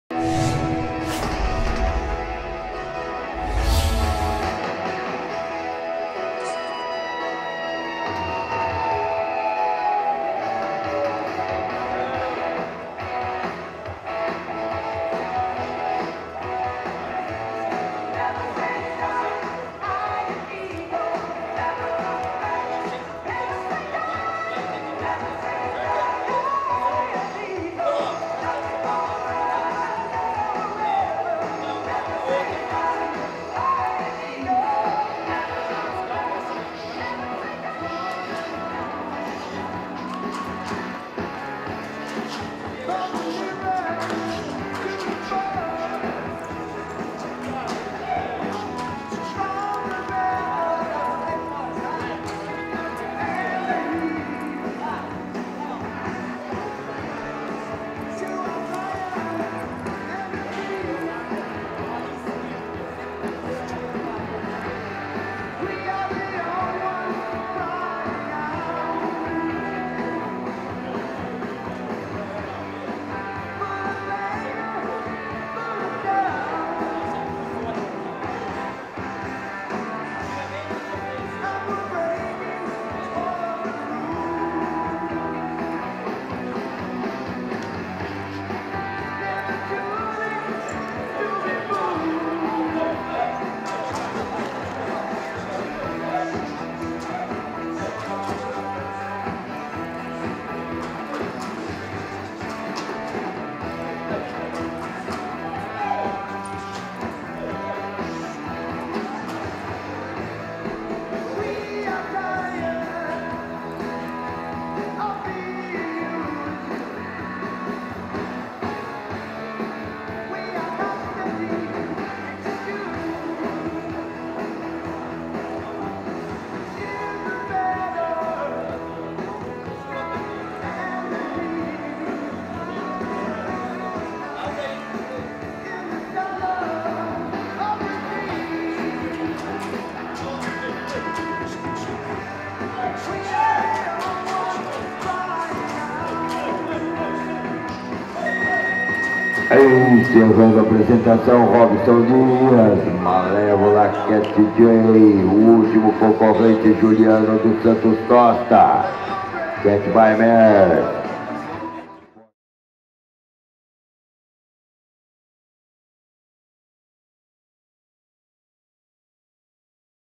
Super Stakes ANCA 2021 - Reservado Campeão Amador
Malevola Cat Jay - Robson Dias
Proprietário: Robson Dias
Filiação: Son Ofa Jay x The Cat Got Loose (High Brow Cat)